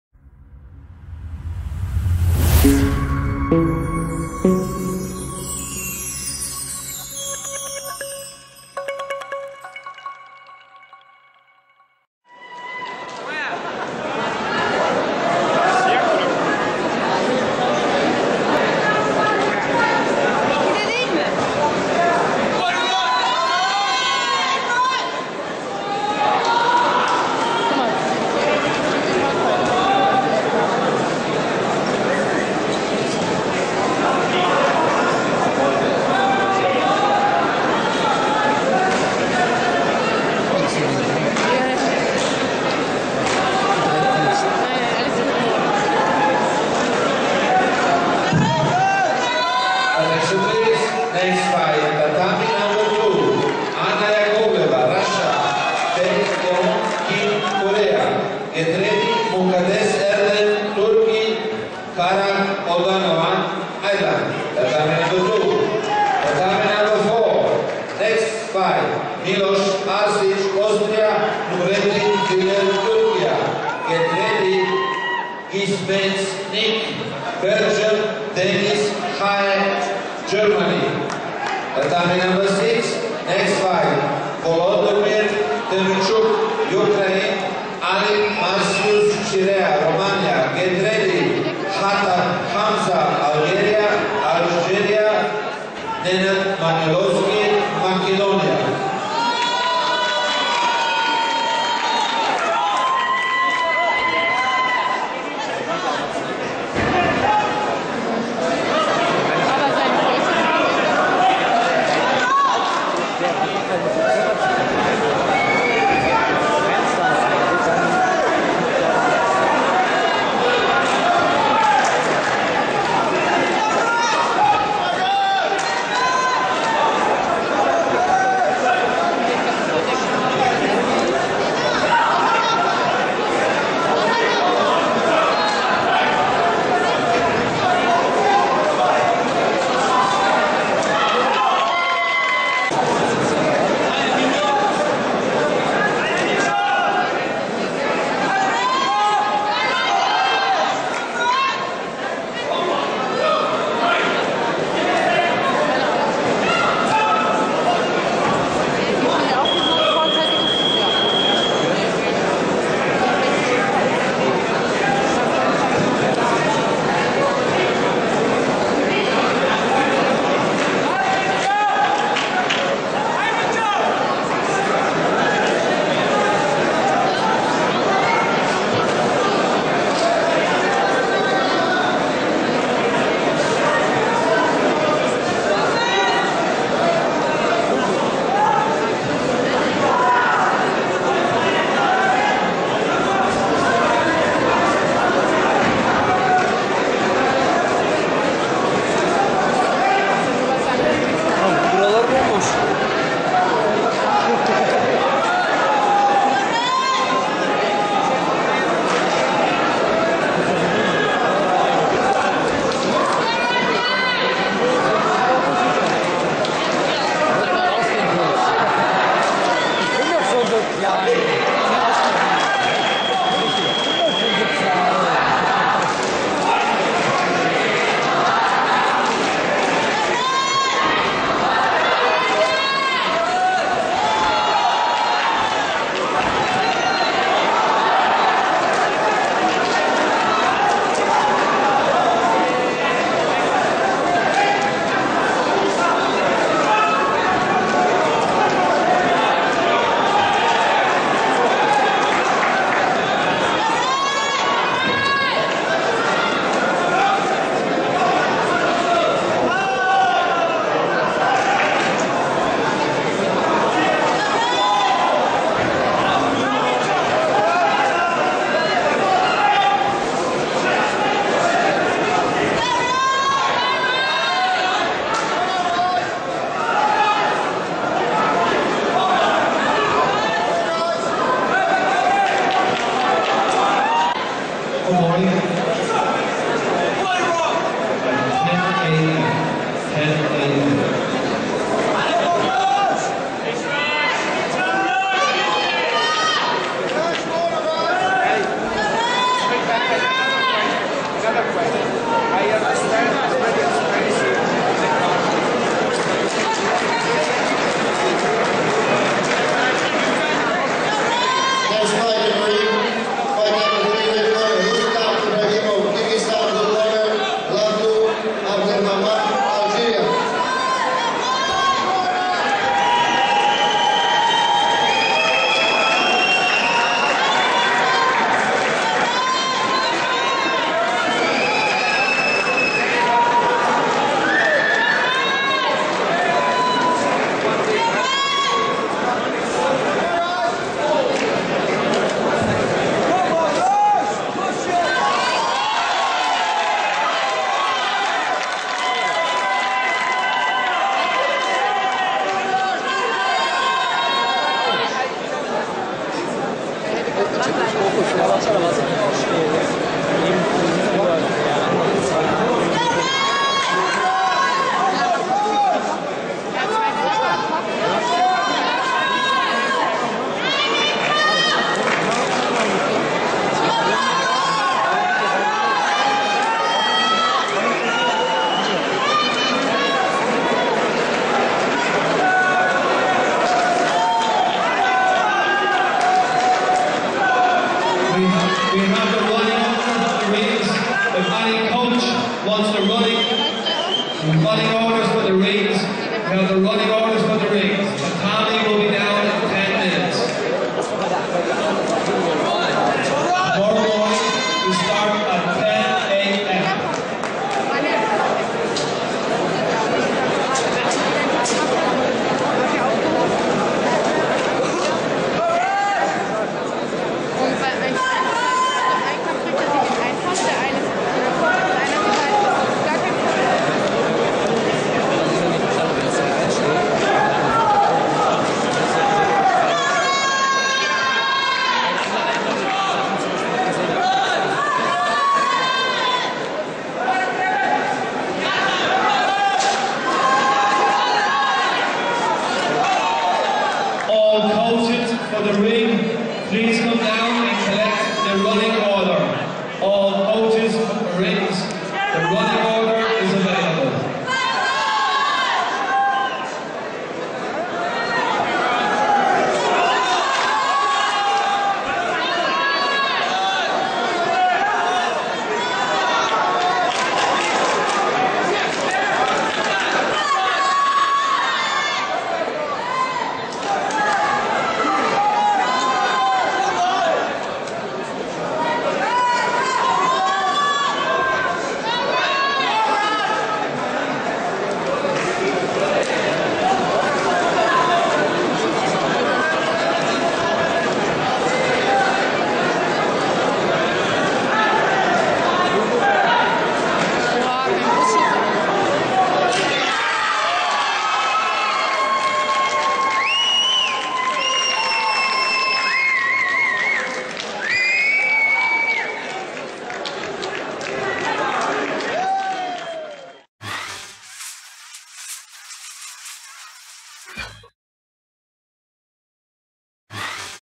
WAKO World Championships Kickboxen Raja Chiripal vs. Mincho Hadjiev
Ein Video über die WAKO World Championships 2013 Antalya, Türkei

Es kämpften im Kickboxen Leichtkontakt Lightcontact -63kg - Raja Chiripal (GBR) vs. Mincho Hadjiev (BUL)